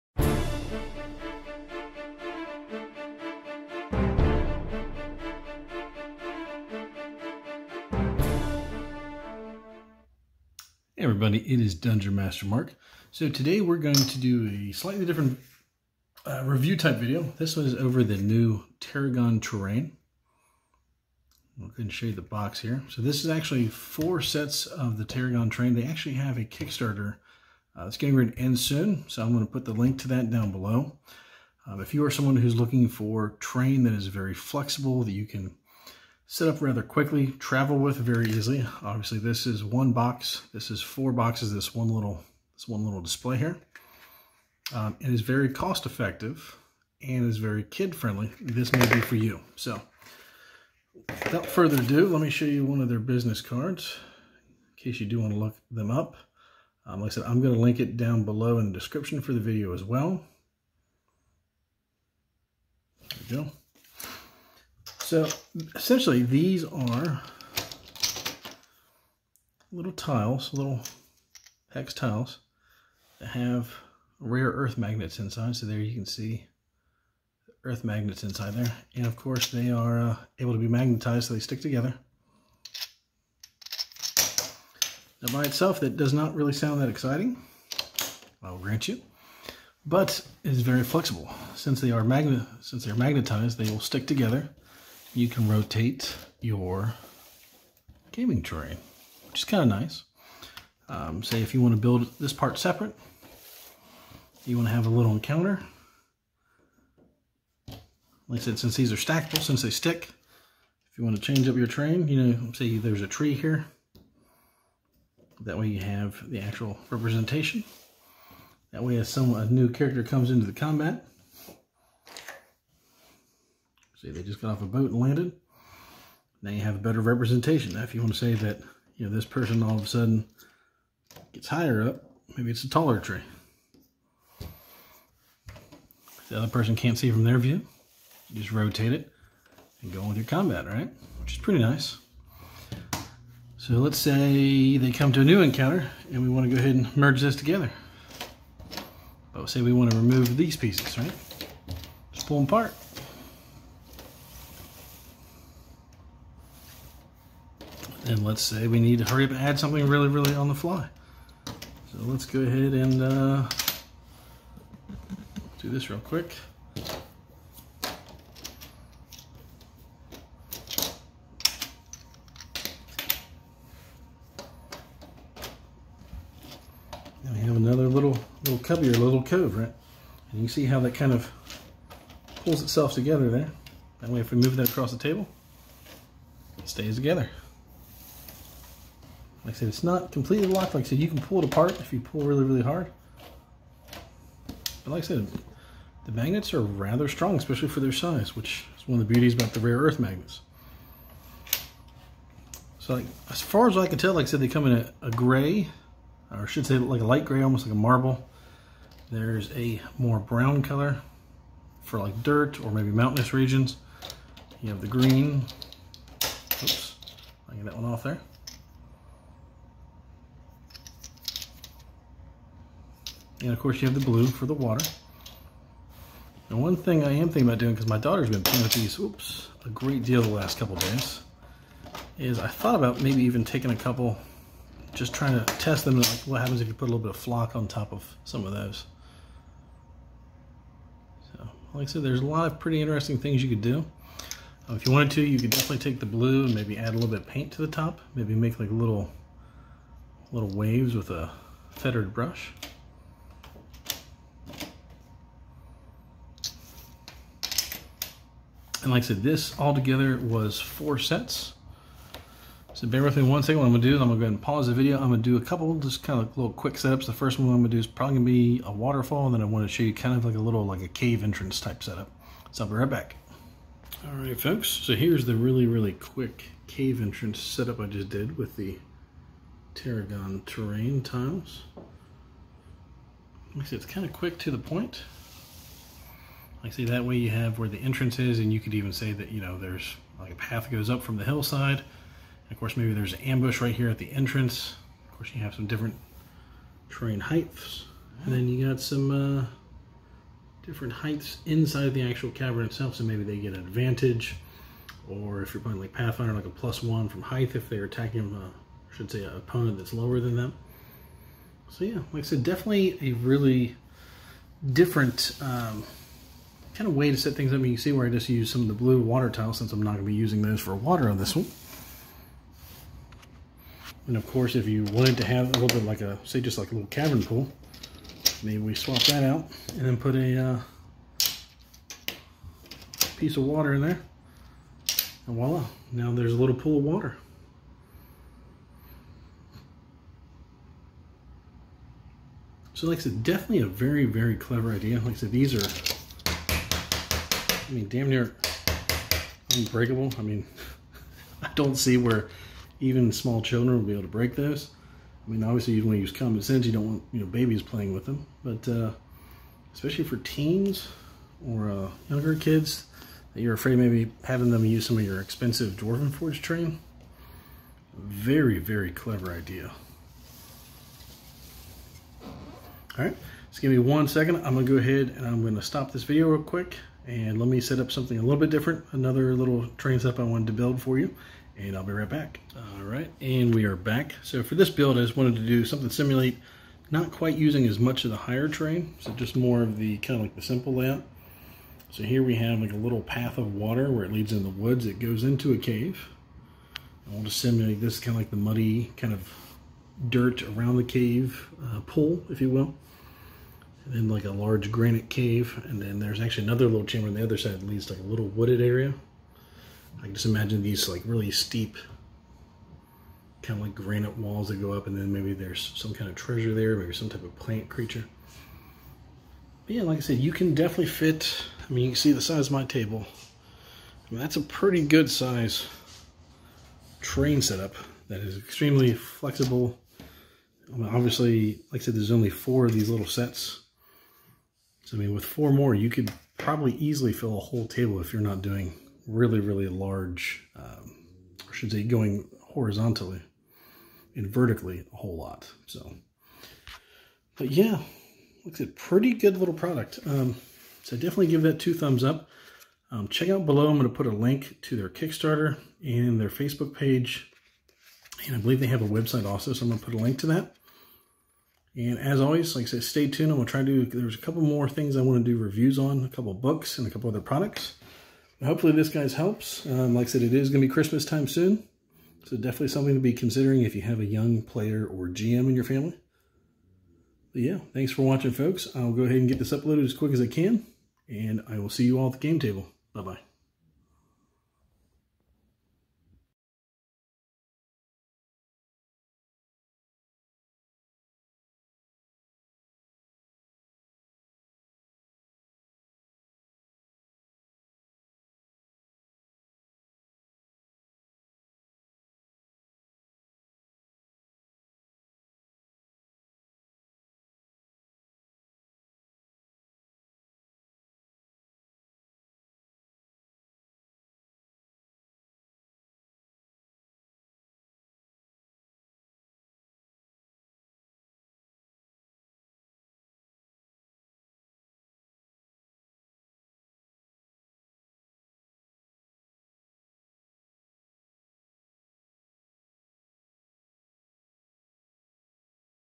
Terragon Terrain( Review )
Terragon Terrain.. its Magnetic and it has a million game warranty ?

If you are looking for Terrain that is Kid friendly , easy and quick to set up( and light)..this may be the product for you. One problem i see alot of Dms handle is bringing terrain with them to a convention. Well this handles the lack of space and the easy setup aspect perfectly.